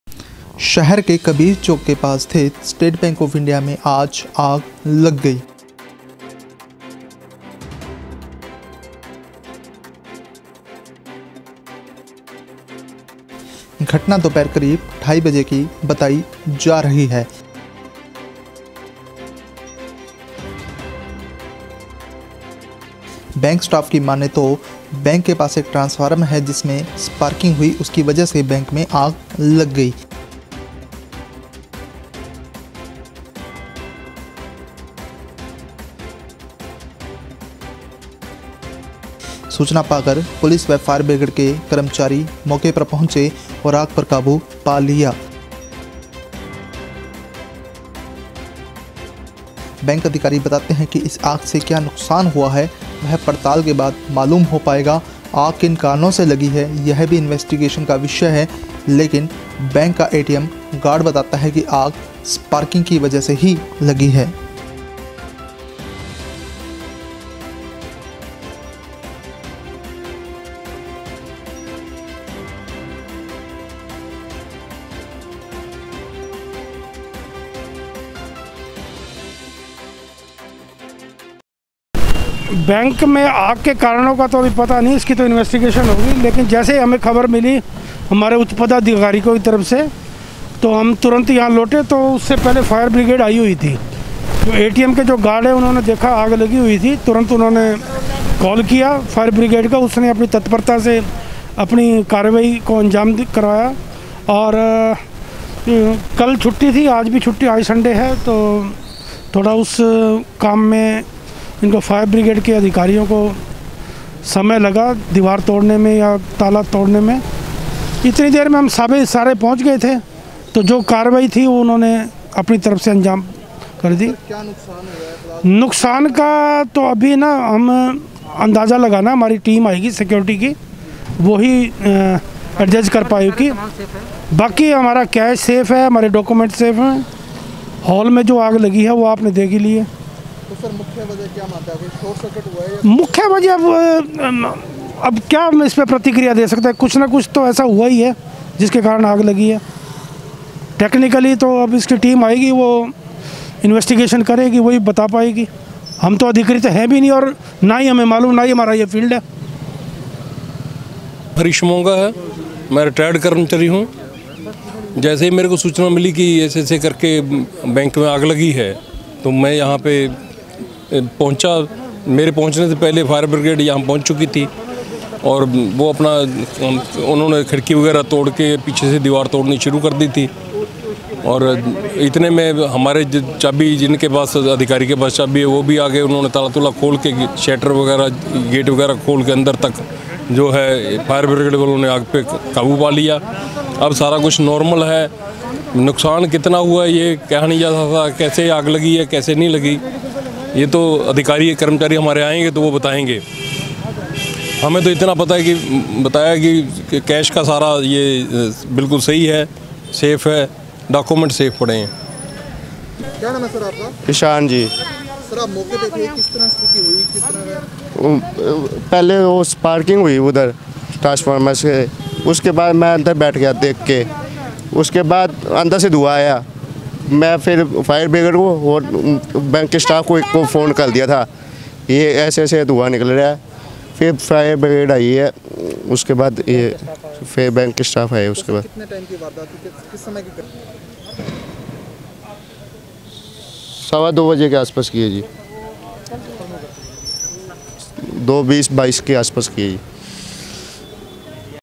सिरसा शहर के एसबीआई बैंक में लगी आग, वजह ये आई सामने
सिरसा शहर के कबीर चौक के पास स्थित स्टेट बैंक ऑफ इंडिया में आज आग लग गई। घटना दोपहर करीब ढाई बजे की है। बैंक स्टॉफ की मानें, तो स्पारकिंग हुई है। सूचना पाकर पुलिस व फायर बिग्रेड की गाड़ी मौके पर पहुंची और आग पर काबू पा लिया है। बैंक अधिकारी बतातें है कि इस आग से क्या नुकसान हुआ है वह पड़ताल के बाद पता लग पाएगा। आग किन कारणों से लगी यह भी इन्वेसिटगेशन के  बाद मालूम होगा लेकिन बैंक का एटीएम गार्ड बताता है कि आग स्पारकिंग से लगी है।